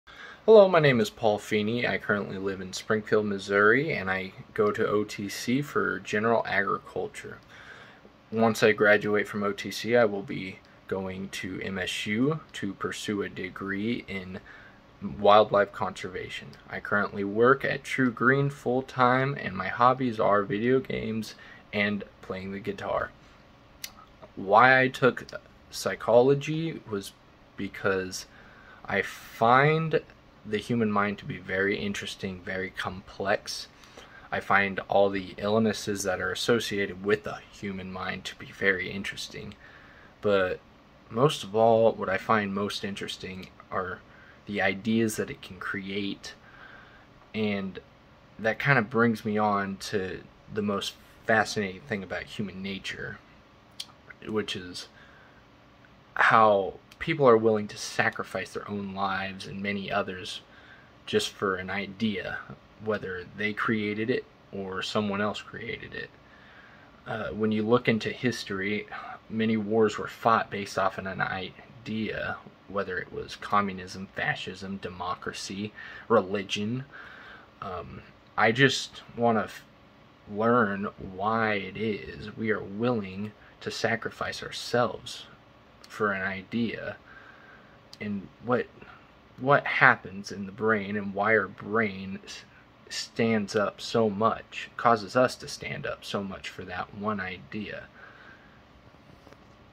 My Introduction
Hello, my name is Paul Feeney I currently live in Springfield Missouri and I go to OTC for general agriculture once I graduate from OTC I will be going to MSU to pursue a degree in wildlife conservation. I work at TruGreen full time, and my hobbies are video games and playing the guitar. Why I took psychology is because I find the human mind to be very interesting, very complex. I find all the illnesses to be associated with the human mind to be very interesting, but most of all what I find most interesting are the ideas it can create, and that kind of brings me on to the most fascinating thing about human nature which is how people are willing to sacrifice their own lives and many others just for an idea whether they created it, or someone else created it. when you look into history many wars were fought based off of an idea whether it was communism, fascism. democracy, religion. I just want to learn why it is we are willing to sacrifice ourselves for an idea, and what happens in the brain, and why our brain causes us to stand up so much for that one idea.